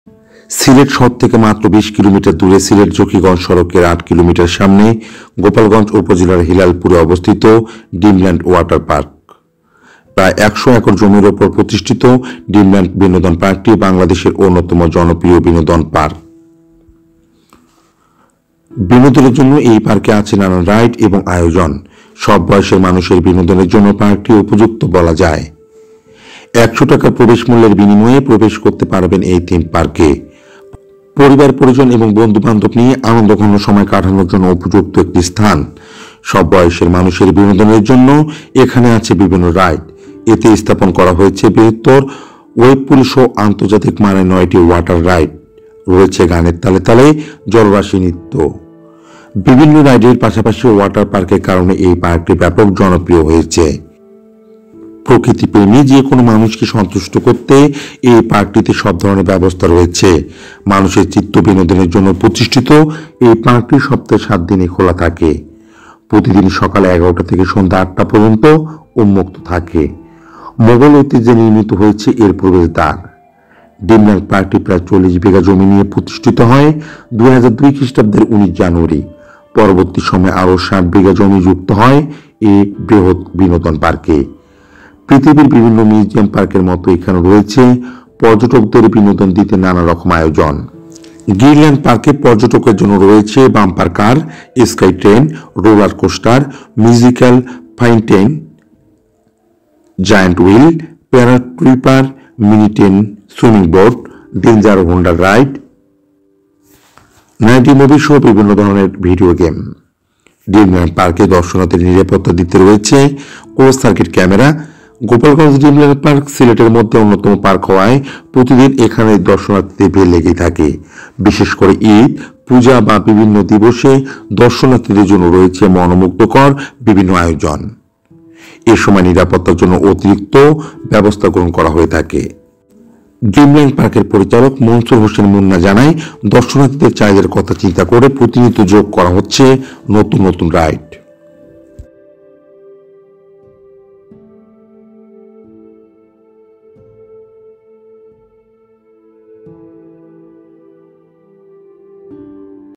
DreamLand Water Park - Sylhet ড্রিমলেন্ড ওয়াটার পার্ক - সিলেট ।
সিলেট শহর থেকে মাত্র 20 কিলোমিটার  দূরে সিলেট-জকিগঞ্জ সড়কের ৮ কিলোমিটার সামনে গোলাপগঞ্জ উপজেলার হিলালপুরে অবস্থিত ড্রিমল্যান্ড ওয়াটার পার্ক।প্রায় ১০০ একর জমির ওপর প্রতিষ্ঠিত ড্রিমল্যান্ড বিনোদন পার্কটি বাংলাদেশের অন্যতম জনপ্রিয় বিনোদন পার্ক।